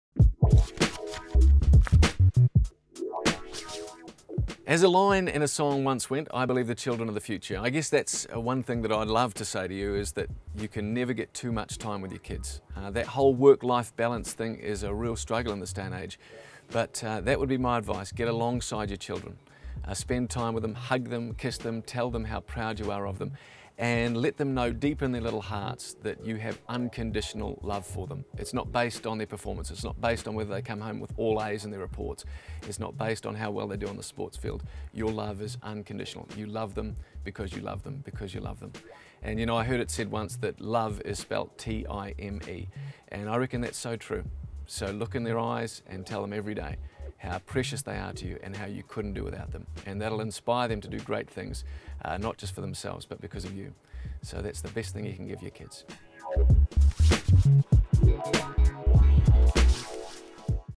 Simon Barnett - Because You Love Them
Simon Barnett is a broadcaster and television personality who first shot to fame as the host of kids TV series What Now. Since then Barnett has hosted a number of high profile TV series including Clash of the Codes and Stars In Their Eyes. He is currently hosting breakfast radio on MORE FM with Gary McCormick, a show he previously hosted with another Rattle Ya Dags speaker, Phil Gifford.